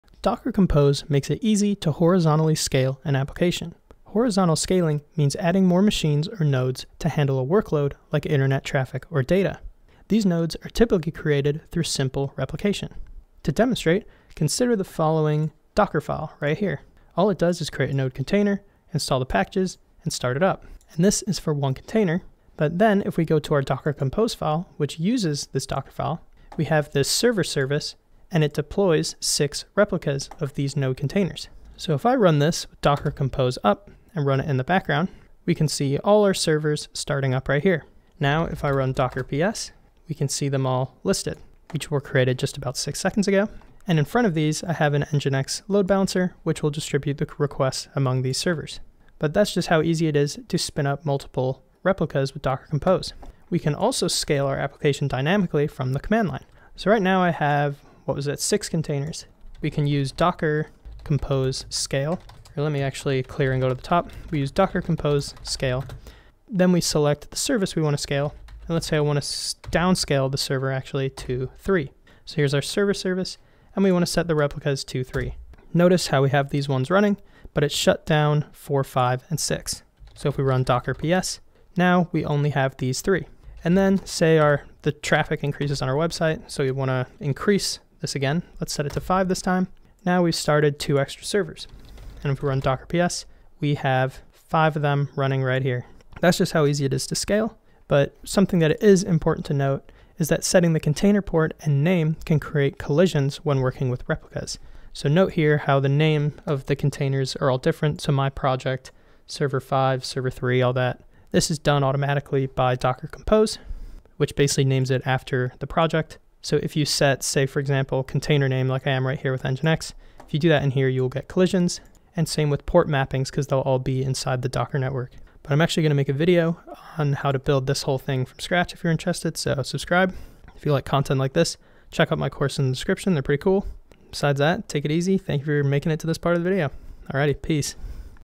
Scaling Services with Docker Compose
Learn how to horizontally scale an application using Docker Compose. To demonstrate, we will spin up multiple Node Docker containers using both the command line and a Docker Compose configuration.

⌛ Timestamps
0:00 - What is Horizontal Scaling?
0:13 - Docker Container Replicas
0:53 - Dynamic Horizontal Scaling
1:37 - Hostname and Port Collisions
2:05 - Outro